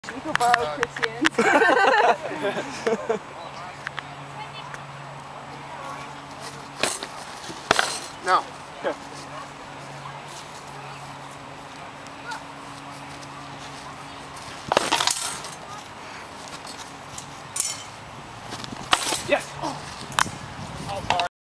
Prima Spada - Miguel de Santiago vs. Lorenzo di Calabria 4
Prima Spada duel for position #9 at Baron Wars XI